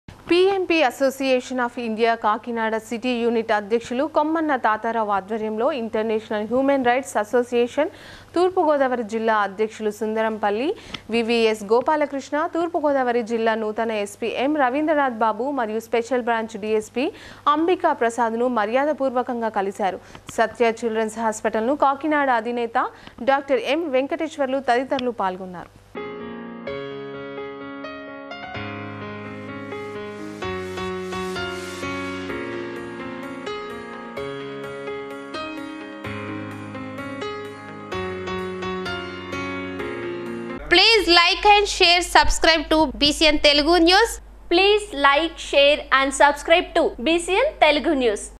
తూర్పుగోదావరి జిల్లా నూతన SPగా M.రవీంద్రనాథ్ బాబు బాధ్యతలు |East Godavari District |BCN Telugu News
పి యమ్ పి అసోసియేషన్ ఆఫ్ ఇండియా కాకినాడ సిటీ యూనిట్ అధ్యక్షులు   కొమ్మన తాతారావు ఆధ్వర్యంలో ఇంటర్నేషనల్ హ్యూమన్ రైట్స్ అసోసియేషన్ తూర్పు గోదావరి జిల్లా అధ్యక్షులు   సుందరపల్లి
వి వి యస్ గోపాలకృష్ణ, తూర్పుగోదావరి జిల్లా నూతన  SP   M.రవీంద్రనాథ్ బాబు, మరియు స్పెషల్ బ్రాంచ్ DSP  అంబికా ప్రసాద్ ను   మర్యాదపూర్వకంగా కలిశారు.సత్య చిల్డ్రన్స్ హాస్పిటల్ కాకినాడ అధినేత Dr. M. వెంకటేశ్వర్లు  తదితరులు పాల్గొన్నారు.